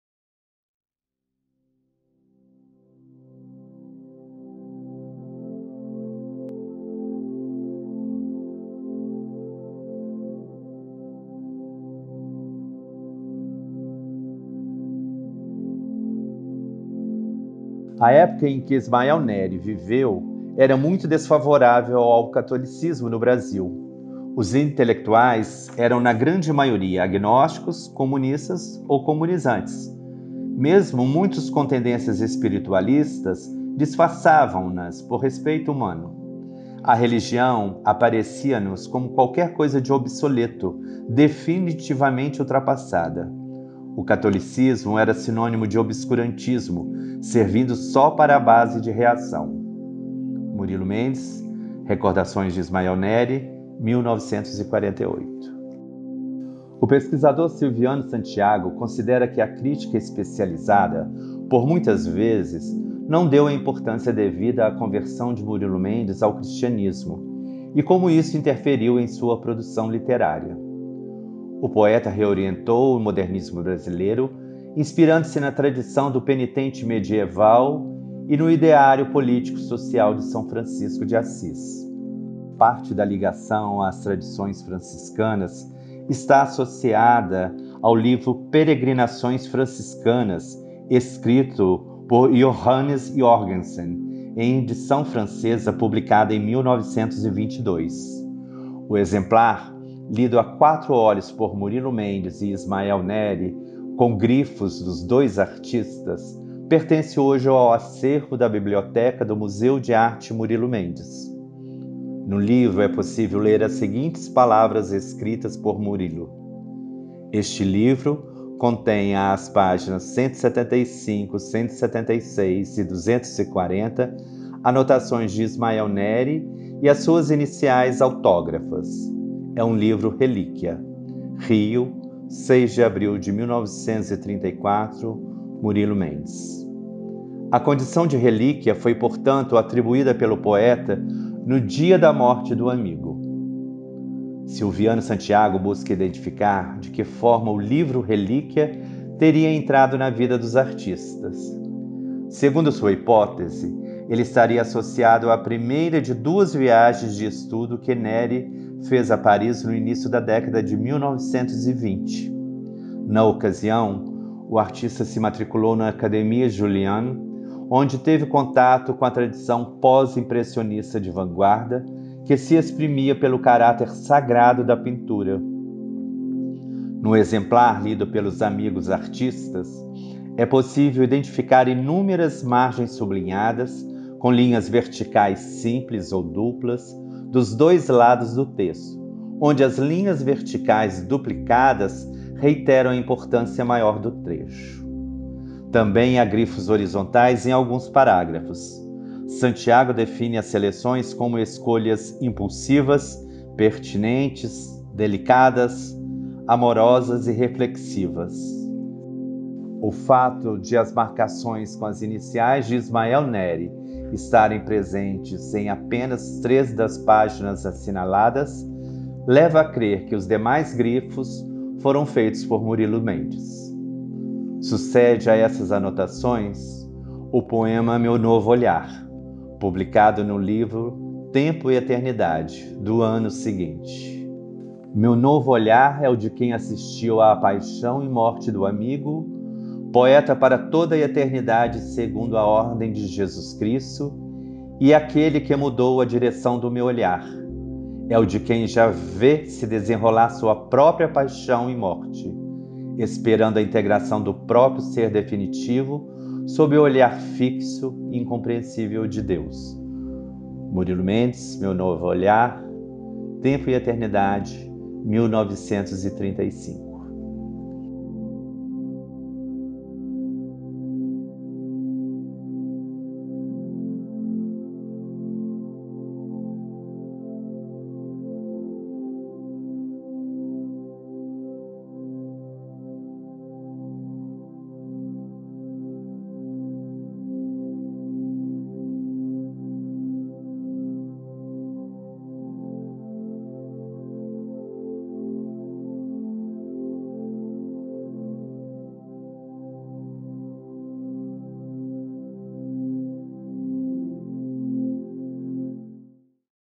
O livro-relíquia | Inspirado no texto de Silviano Santiago
Continuando as comemorações do centenário de amizade entre Murilo Mendes e Ismael Nery, o MAMM apresenta "O livro-relíquia".

Disponível em vídeo e podcast, o conteúdo é inspirado no texto "Poesia fusão: catolicismo primitivo/mentalidade moderna" do pesquisador Silviano Santiago, publicado como posfácio do livro Poemas, de Murilo Mendes, pela editora Cosac Naify. Nele é abordado a relação dos amigos com o catolicismo, especialmente com as tradições franciscanas, por meio da análise do livro Pélerinages Franciscains de Johannes Joergensen.

"O livro-relíquia" também está disponível em podcast no canal do Museu de Arte Murilo Mendes no Spotify.